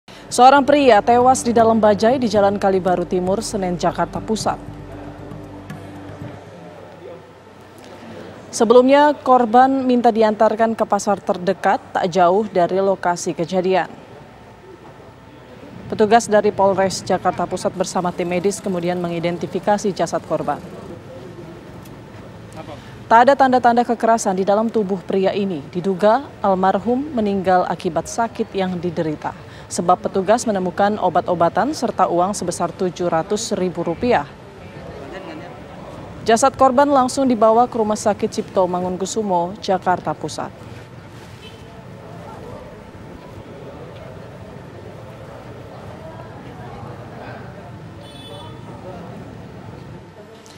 Seorang Pria Tewas di Dalam Bajaj di Jalan Kali Baru Timur Senen Jakpus
Jakarta, tvOnenews.com - Seorang Pria Tewas di Dalam Sebuah Bajaj di Jalan Kali Baru Timur Senen Jakpus